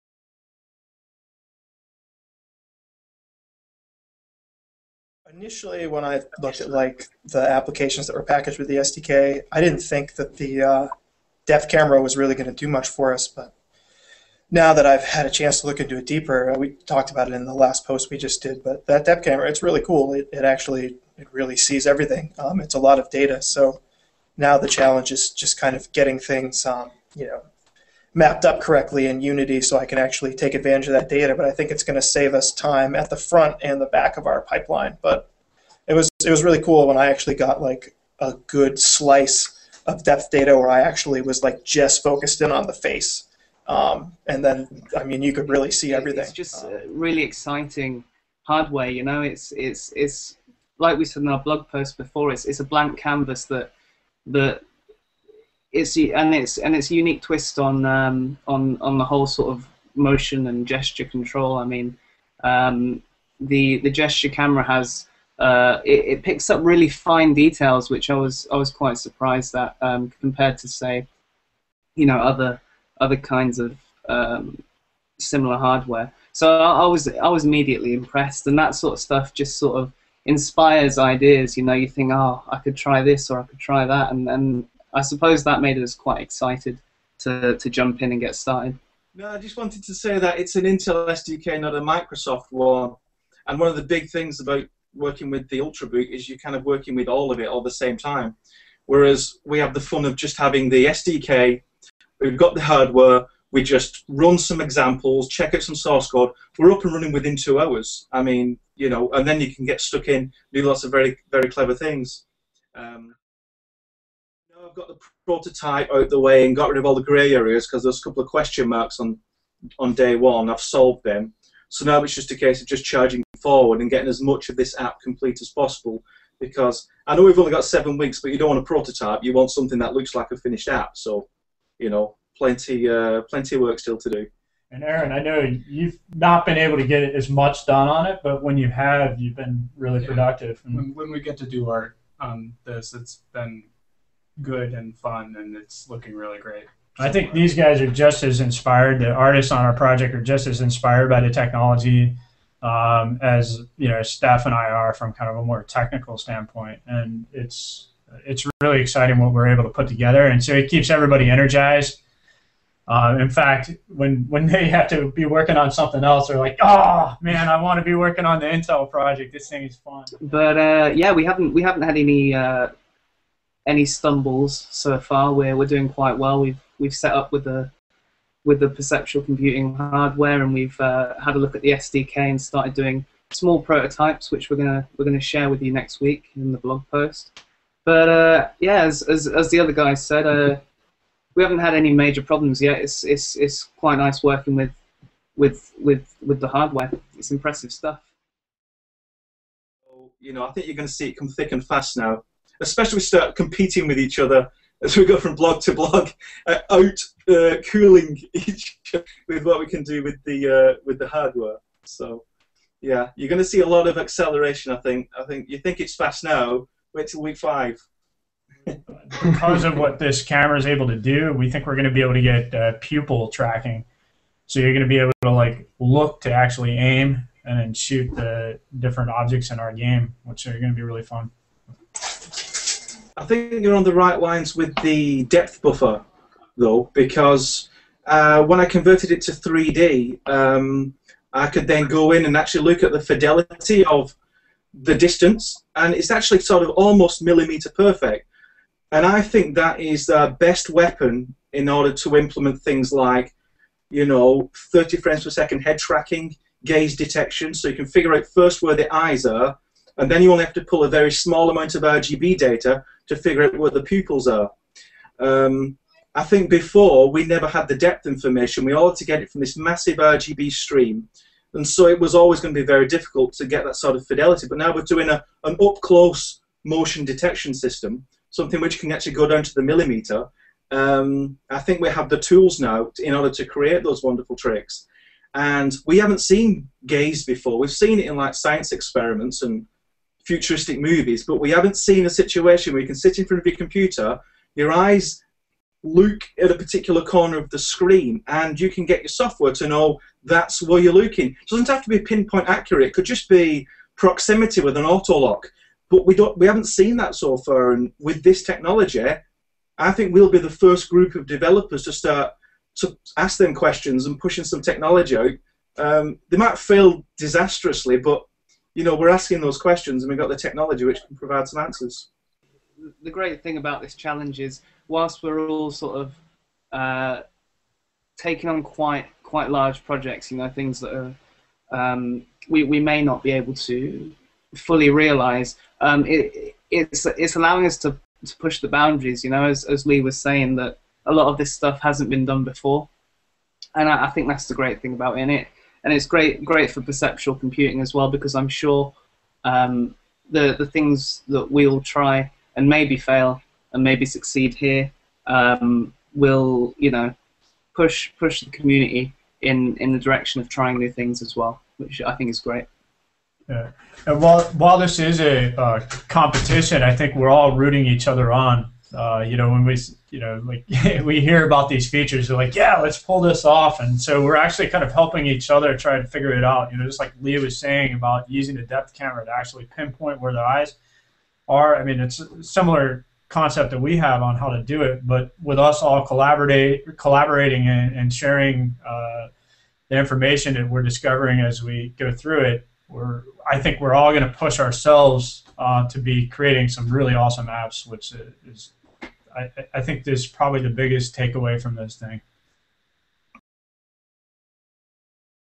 Ultimate Coder Challengers: Going Perceptual, Week 2
During week 2 of the Ultimate Coder Challenge, we take time to talk with developers who are using the Intel Perceptual Computing SDK to build apps that can track hand and head gestures to control applications.
YouTube Editor version of a longer Google Hangout discussion